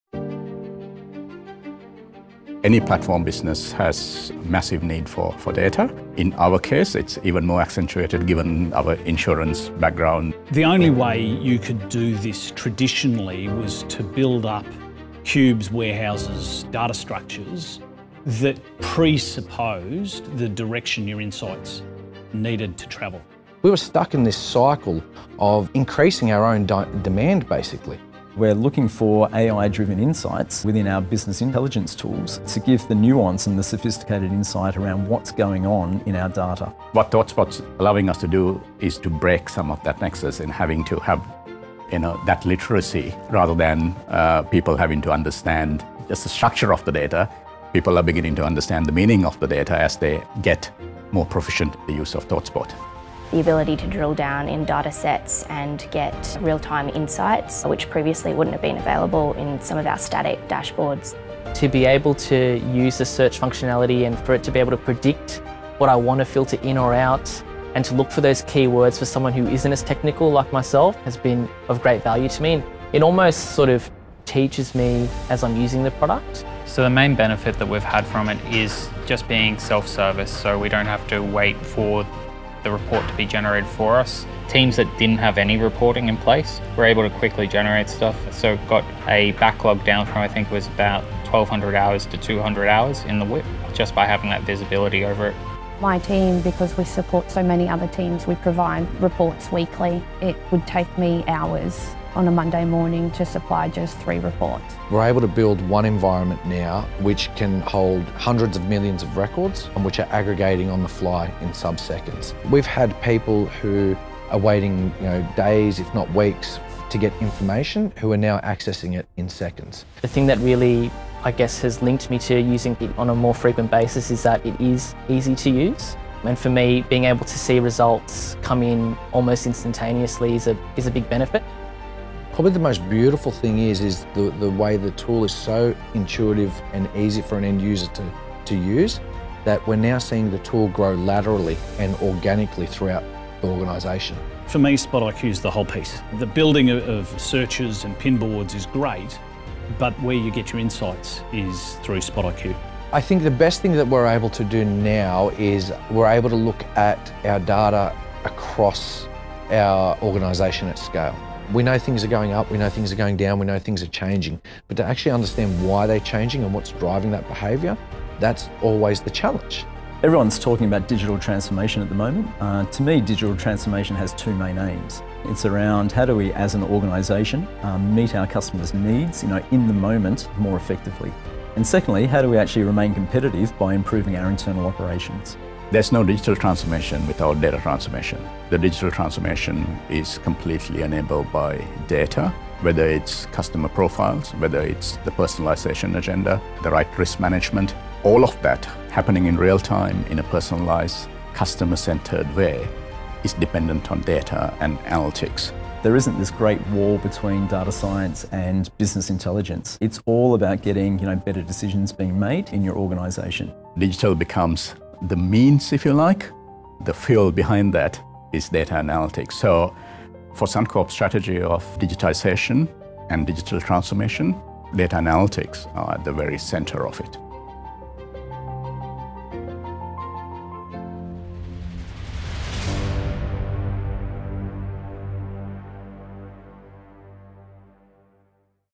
Suncorp + ThoughtSpot: Transforming Claims Operations with Search and AI
Suncorp,  one of Australia’s largest insurance and banking corporations, is fueling their digital transformation initiative with search and AI-driven analytics. With ThoughtSpot, Suncorp users across the organization can explore complex data questions and make smarter business decisions in the moment that allow Suncorp to meet their customers' needs more effectively and remain competitive by improving internal operations.